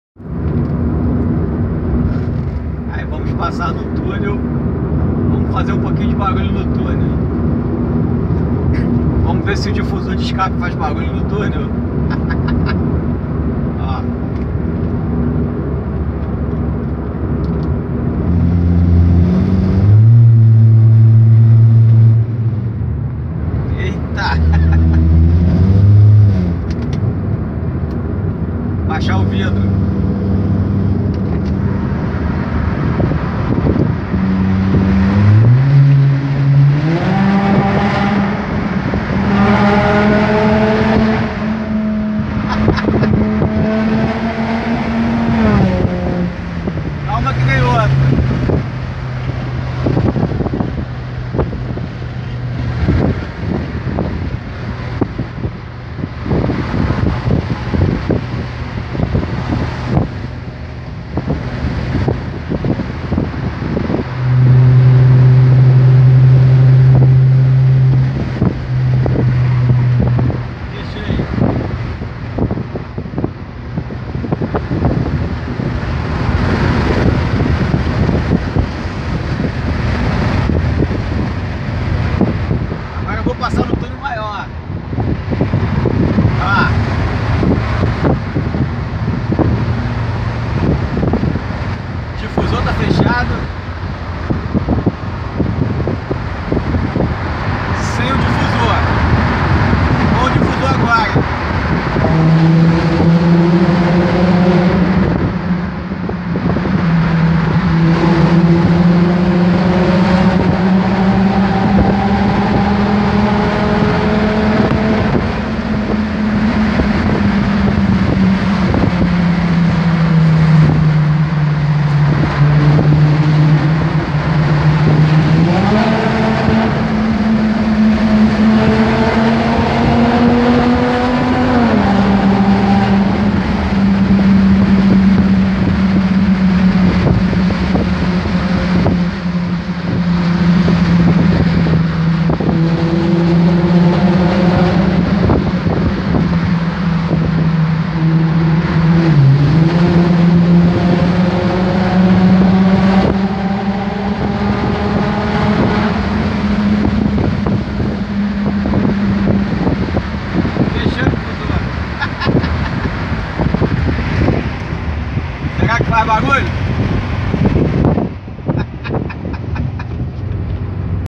Jetta 2.5 com difusor de escape no túnel.
Fazendo muito barulho no túnel da linha Amarela Rio de Janeiro.👍✌